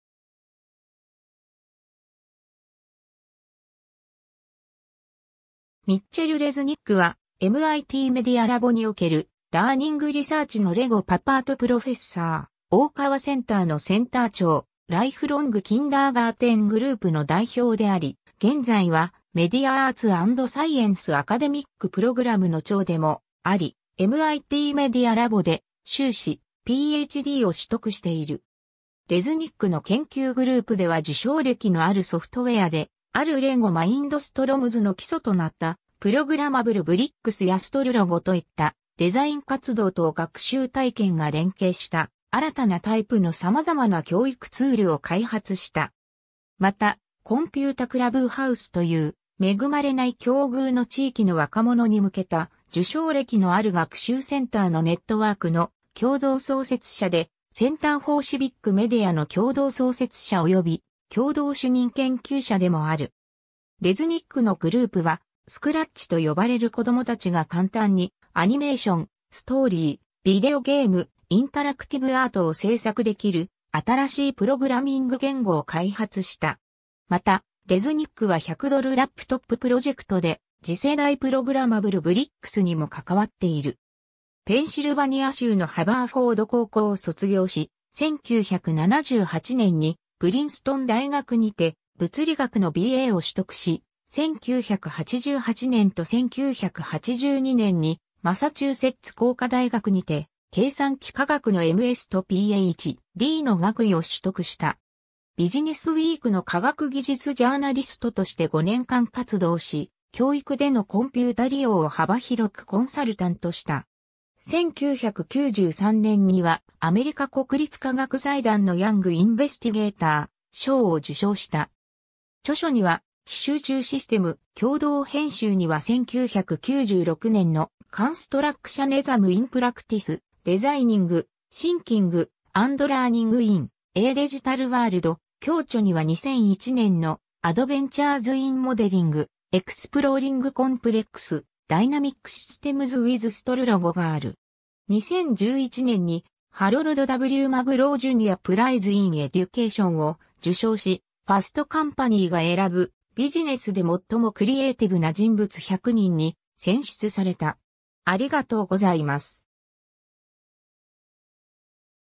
ミッチェル・レズニック
ミッチェル・レズニック（英: Mitchel Resnick、1956年6月12日 - ）は、MITメディアラボにおけるラーニング・リサーチのLEGOパパート・プロフェッサー、大川センターのセンター長、ライフロング・キンダーガーテン・グループの代表であり、現在はメディア・アーツ・アンド・サイエンス・アカデミック・プログラムの長でもあり、MITメディアラボで修士、Ph.D.を取得している。
レズニックの研究グループでは受賞歴のあるソフトウェアであるLEGO Mindstormsの基礎となった「プログラマブル・ブリックス」やStarLogoといった、デザイン活動と学習体験が連携した新たなタイプの様々な教育ツールを開発した。
また、コンピュータ・クラブハウスという恵まれない境遇の地域の若者に向けた受賞歴のある学習センターのネットワークの共同創設者で、Center for Civic Mediaの共同創設者及び共同主任研究者でもある。
 レズニックのグループはScratchと呼ばれる子供達が簡単にアニメーション、ストーリー、ビデオゲーム、インタラクティブアートを製作できる新しいプログラミング言語を開発した。
また、レズニックは100ドルラップトッププロジェクトで次世代プログラマブル・ブリックスにも関わっている。
 ペンシルバニア州のハヴァーフォード高校を卒業し、1978年にプリンストン大学にて物理学のBAを取得し、1988年と1982年にマサチューセッツ工科大学にて計算機科学のMSとPh.Dの学位を取得した。
ビジネスウィークの科学技術ジャーナリストとして5年間活動し、教育でのコンピュータ利用を幅広くコンサルタントした。
1993年にはアメリカ国立科学財団のヤング・インベスティゲーター賞を受賞した。
著書には「非集中システム」、共同編集には1996年の「Constructionism in Practice: Designing, Thinking, and Learning in a Digital World」、共著には2001年の「Adventures in Modeling: Exploring Complex, Dynamic Systems with StarLogo」がある。
 2011年に、ハロルド・W・マグロー・ジュニア・プライズ・イン・エデュケーションを受賞し、ファスト・カンパニーが選ぶ、ビジネスで最もクリエイティブな人物100人に選出された。